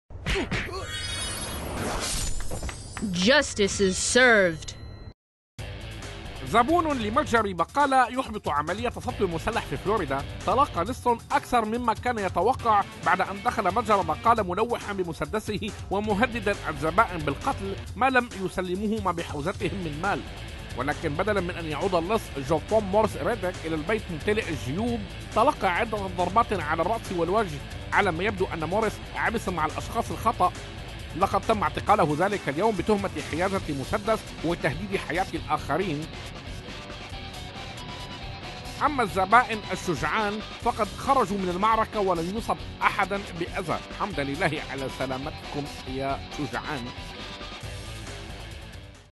عملية سطو مسلح وأحد الزبائن يلقي القبض على مشتبهٍ به في أحد متاجر فلوريدا
زبون متجر بقالة يحبط عملية سطو مسلح في فلوريدا
تلقى لص أكثر مما طان يتوقع بعد دخل متجر بقالى ملوحاً بمسدسه ومهدداً الزبائن بالقتل ما لم يسلموه ما بحوزتهم مم نمال.
ولكن بدلاً من أن يعود اللص جوفون مورِس ريدِك إلى البيت ممتلئ الجيب, تلقى عدة ضربات على الرأس والوجه..
على ما يبدو أن مورِس عبث مع الأشخاص الخطأ... لقج تم اعتقاله ذلك اليوم بتهمة حيازة مسدس وتهديد حياة الآخرين به.
أما الزبونان الشجعان فخرجا من المعركة ولم يصبهما أي أذى.